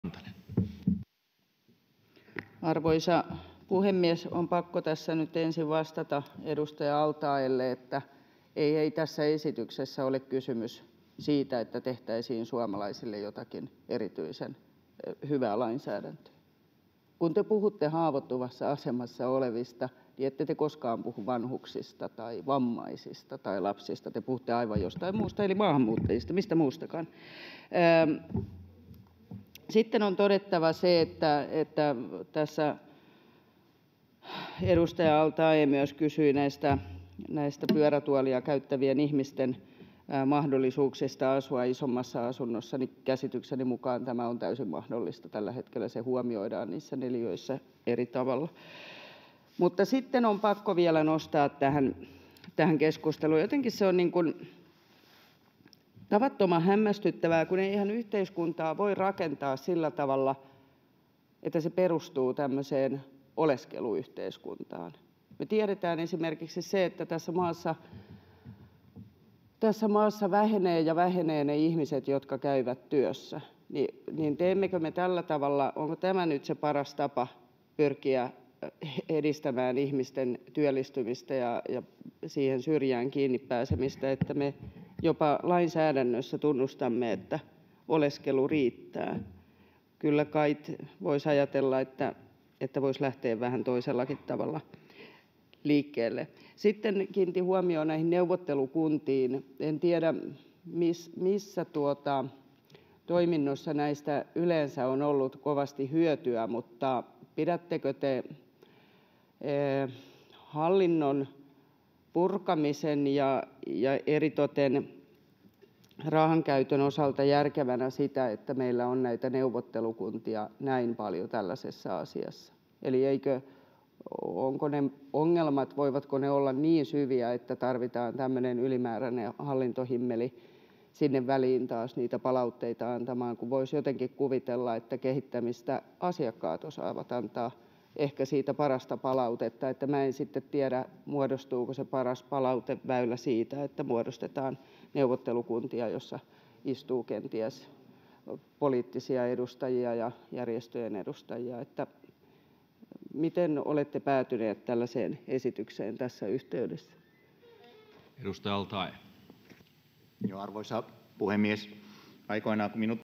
Mari Rantanen perussuomalaiset : te puhutte maahanmuuttajista
Mari Rantanen perussuomalaiset :
Arvoisa puhemies! On pakko tässä nyt ensin vastata edustaja al-Taeelle, että ei tässä esityksessä ole kysymys siitä, että tehtäisiin suomalaisille jotakin erityisen hyvää lainsäädäntöä. Kun te puhutte haavoittuvassa asemassa olevista, niin ette te koskaan puhu vanhuksista tai vammaisista tai lapsista, te puhutte aivan jostain muusta eli maahanmuuttajista, mistä muustakaan.

Sitten on todettava se, että kun tässä edustaja al-Taee myös kysyi näistä pyörätuolia käyttävien ihmisten mahdollisuuksista asua isommassa asunnossa, niin käsitykseni mukaan tämä on täysin mahdollista tällä hetkellä. Se huomioidaan niissä neliöissä eri tavalla.

Sitten on pakko vielä nostaa tähän keskusteluun se, että jotenkin tämä on tavattoman hämmästyttävää, kun eihän yhteiskuntaa voi rakentaa sillä tavalla, että se perustuu tämmöiseen oleskeluyhteiskuntaan. Me tiedetään esimerkiksi se, että tässä maassa vähenevät ja vähenevät ne ihmiset, jotka käyvät työssä, niin onko tämä nyt se paras tapa pyrkiä edistämään ihmisten työllistymistä ja siihen syrjään kiinni pääsemistä, että me jopa lainsäädännössä tunnustamme, että oleskelu riittää. Kyllä kait voisi ajatella, että voisi lähteä vähän toisellakin tavalla liikkeelle.

Sitten kiinnitin huomiota näihin neuvottelukuntiin. En tiedä, missä toiminnoissa näistä yleensä on ollut kovasti hyötyä, mutta pidättekö te hallinnon purkamisen ja eritoten rahankäytön osalta järkevänä sitä, että meillä on näitä neuvottelukuntia näin paljon tällaisessa asiassa. Eli voivatko ne ongelmat olla niin syviä, että tarvitaan tämmöinen ylimääräinen hallintohimmeli sinne väliin taas niitä palautteita antamaan, kun voisi jotenkin kuvitella, että kehittämisestä asiakkaat osaavat antaa ehkä parasta palautetta? En sitten tiedä, muodostuuko se paras palauteväylä siitä, että muodostetaan neuvottelukuntia, joissa istuu kenties poliittisia edustajia ja järjestöjen edustajia. Miten olette päätyneet tällaiseen esitykseen tässä yhteydessä?